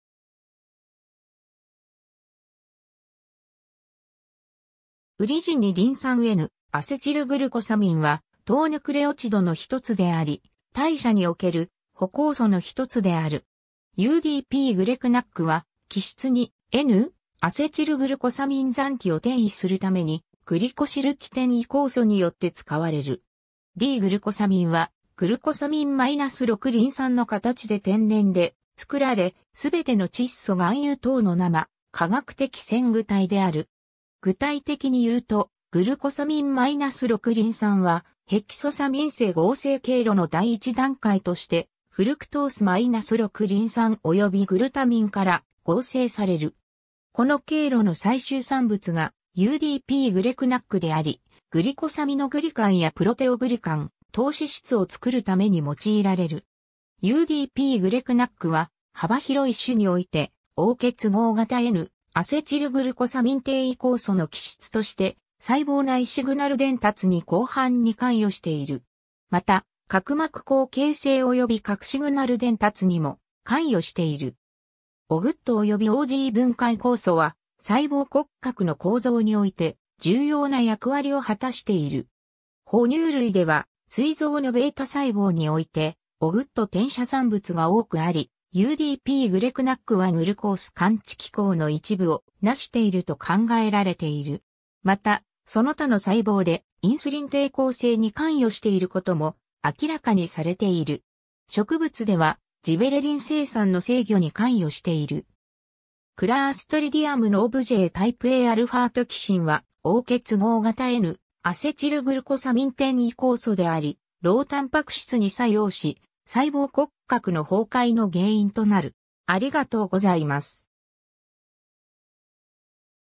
ウリジン二リン酸-N-アセチルグルコサミン
ウリジン二リン酸 N-アセチルグルコサミン（ウリジンにリンさん N-アセチルグルコサミン、Uridine diphosphate N-acetylglucosamine、略称: UDP-GlcNAc）は、糖ヌクレオチドの一つであり、代謝における補酵素の一つである。
UDP-GlcNACは基質にN-アセチルグルコサミン残基を転移するためにグリコシル基転移酵素によって使われる。
D-グルコサミンは、グルコサミン-6-リン酸の形で天然で作られ、全ての窒素含有糖の生化学的前駆体である。
具体的に言うと、グルコサミン-6-リン酸は、ヘキソサミン生合成経路の第一段階としてフルクトース-6-リン酸およびグルタミンから合成される。
この経路の最終産物がUDP-GlcNAcであり、グリコサミノグリカンやプロテオグリカン、糖脂質を作るために用いられる。
 UDP-GlcNAcは、幅広い種においてO結合型N-アセチルグルコサミン転移酵素 (OGT) の基質として細胞内シグナル伝達に広範に関与している。
また、核膜孔形成および核シグナル伝達にも関与している。
OGTおよびOG分解酵素は、細胞骨格の構造において重要な役割を果たしている。
ほ乳類では、膵臓のβ細胞においてOGT転写産物が多くあり、UDP-GlcNAcはグルコース感知機構の一部をなしていると考えられている。
また、その他の細胞でインスリン抵抗性に関与していることも明らかにされている。
植物では、ジベレリン生産の制御に関与している。
 Clostridium novyiタイプAαトキシンは、O結合型N-アセチルグルコサミン転移酵素であり、Rhoタンパク質に作用し、細胞骨格の崩壊の原因となる。